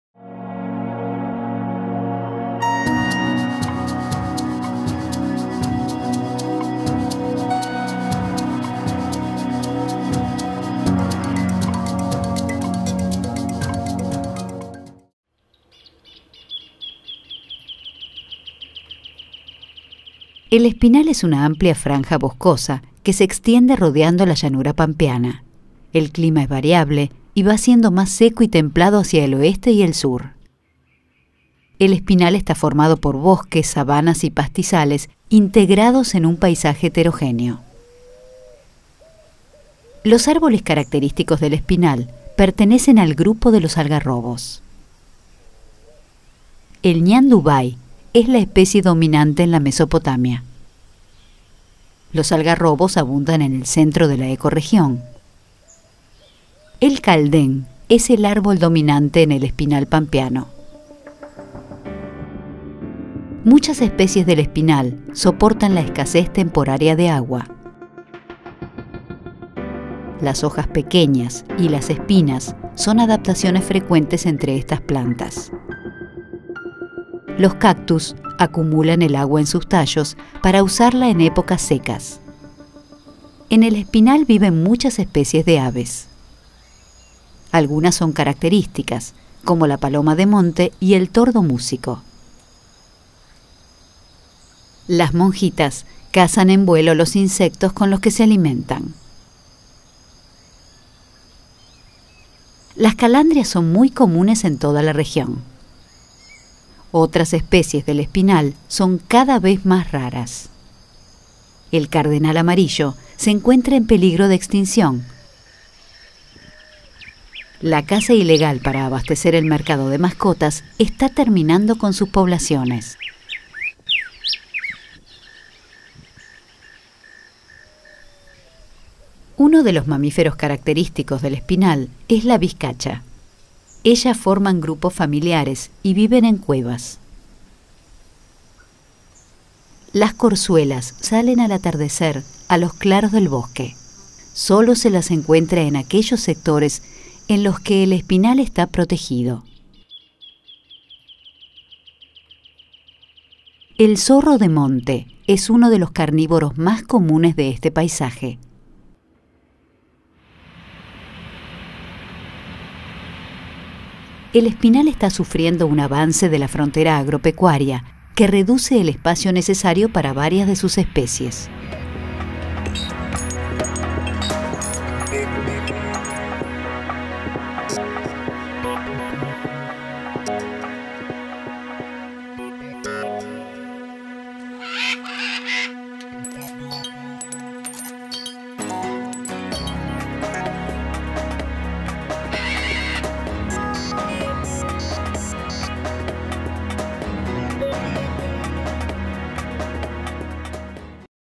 Ecorregiones - Espinal
Serie de micro programas realizada para la fundación Azara que resume las características de las principales ecorregiones de la Argentina.
Idea: Adrián Giacchino
Producción: Clarice Neves Francisco
Guión y dirección: Marcelo Viñas
Narración: Laura Sordi
Sonido: José Luis Pogacnik
Productora: Timbó films
Año: 2015